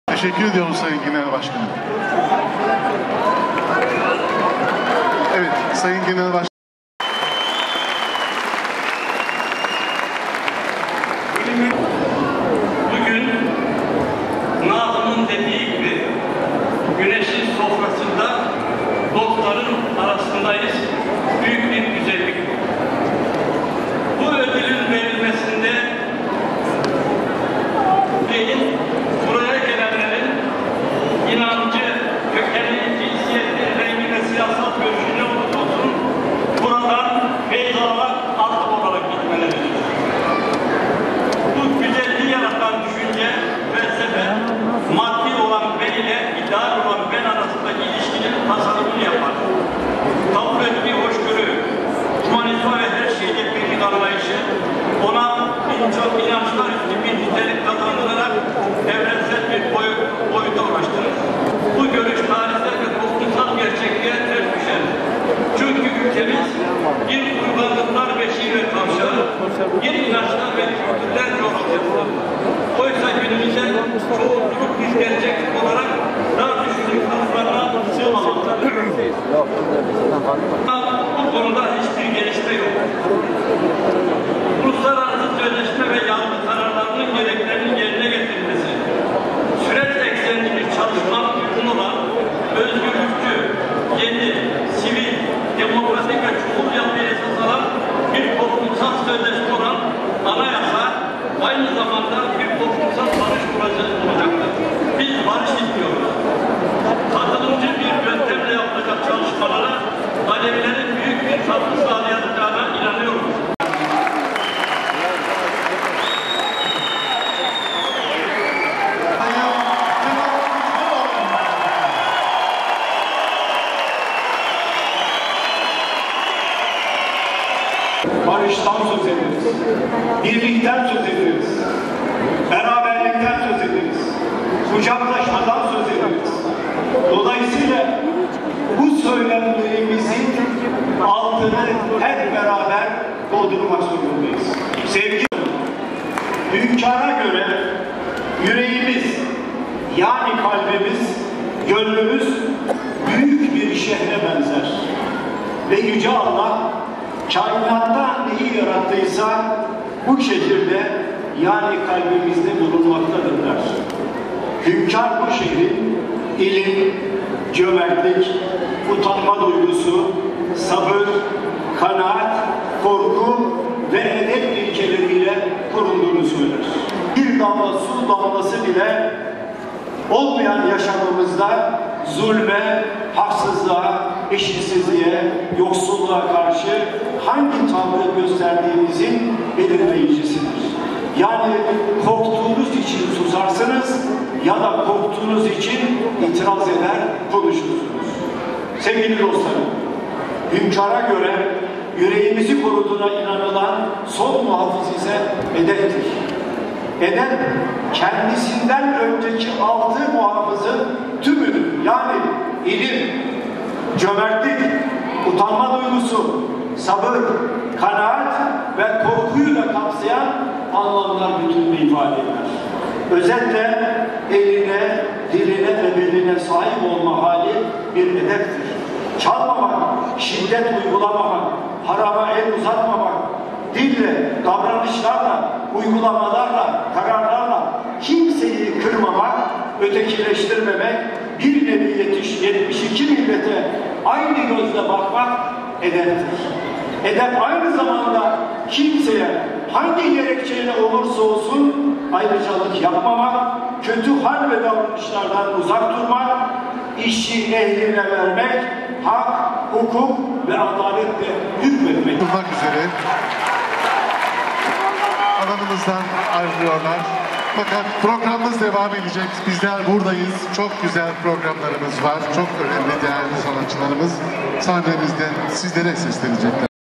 29. Hacı Bektaş Veli Dostluk ve Barış Ödülü Kamil Ateşoğulları’na verildi
PİRHA-59. Ulusal, 33. Uluslararası Hacı Bektaş Veli Anma Törenleri ve Kültür Sanat Etkinlikleri kapsamında bu yıl 29’uncusu verilen Hacı Bektaş Veli Dostluk ve Barış Ödülü, Yazar Kamil Ateşoğulları’na verildi.